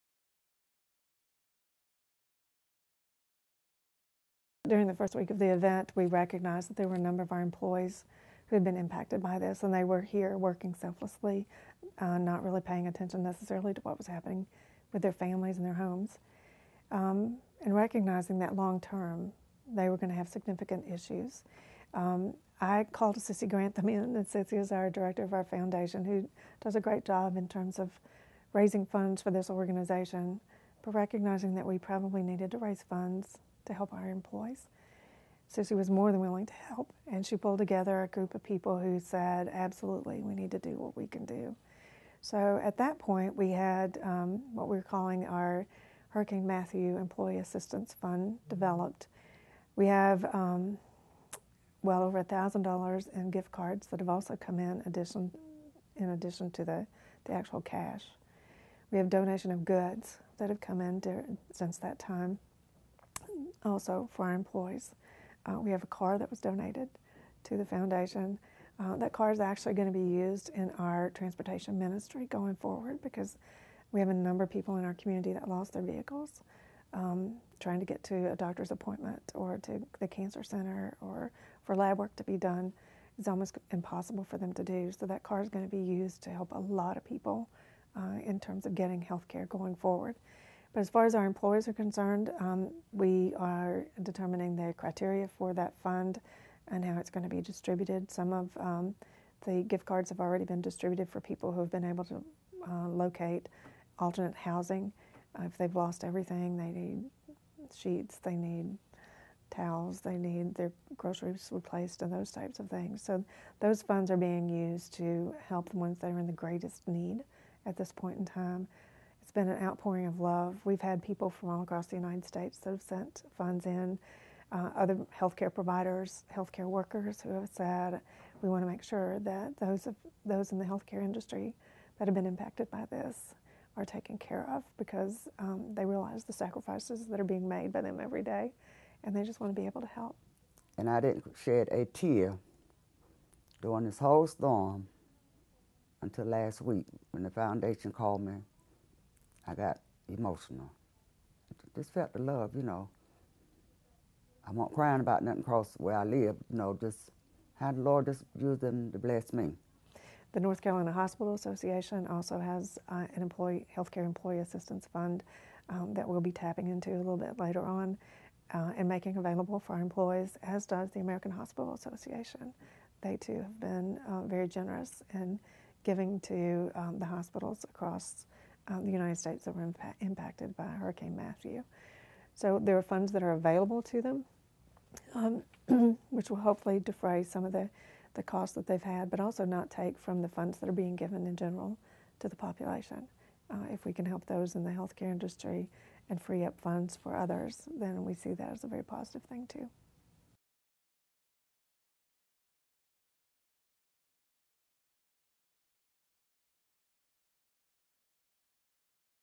SeHealth Hurricane Matthew Employee Relief Fund
SeHealth President/CEO Joann Anderson explains the fund, its creation and mission. Food Services Gladys Scott describes how touched she was by the relief team's care and concern for her well-being following Hurricane Matthew.